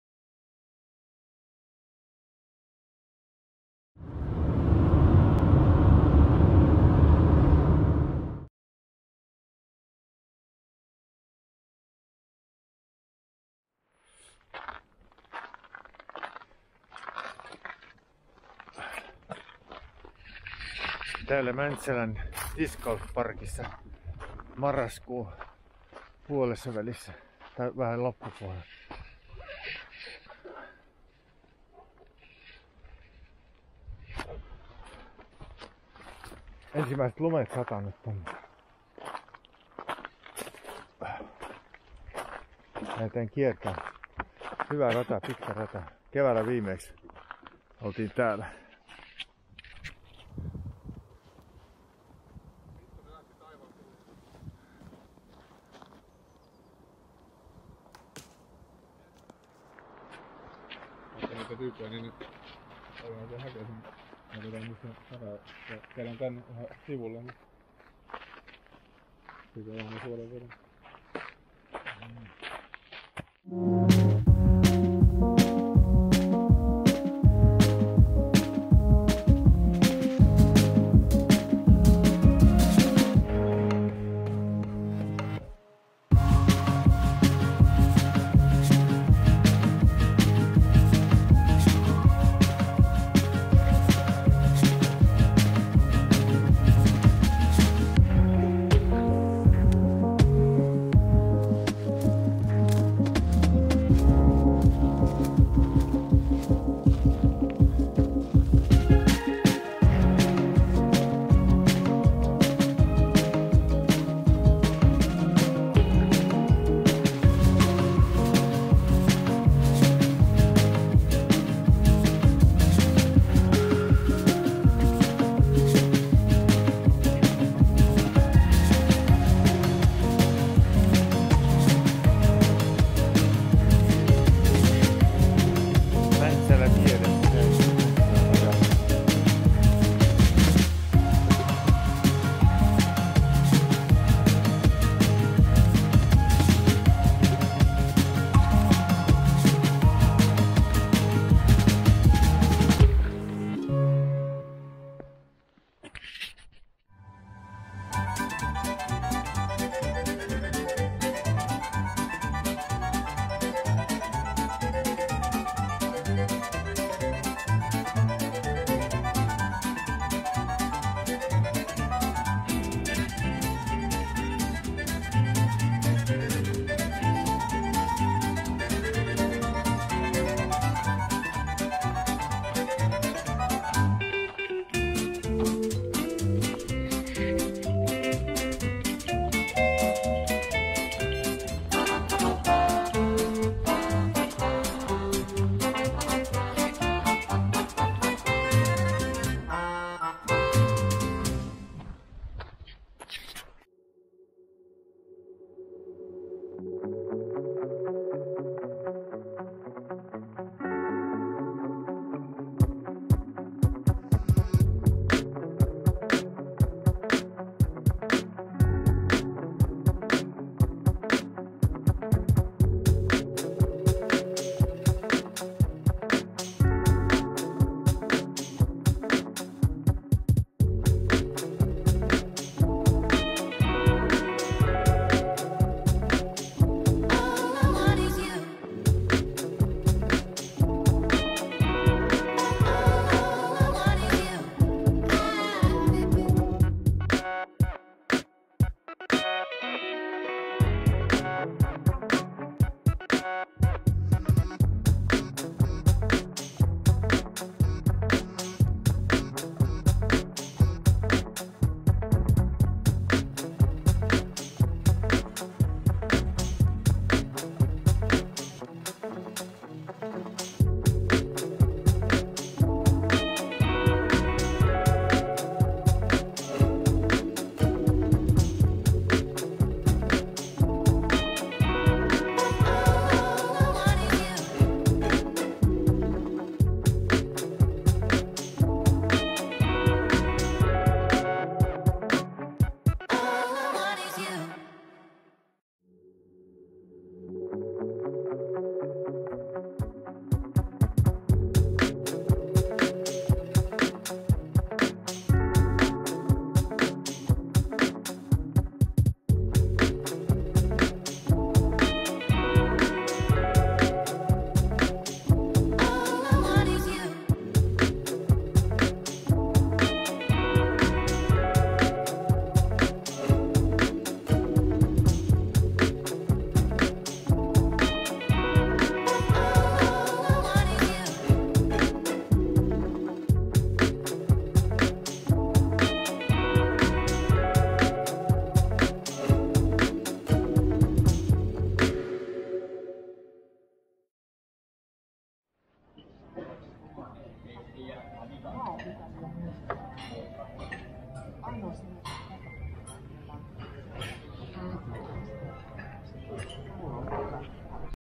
Mäntsälä DiscGolfPark marraskuussa 2020 Kari ja Samuli testaa.
Kari ja Samuli kiersivät ensilumen sataman Mäntsälän mainion frisbeegolfrafan.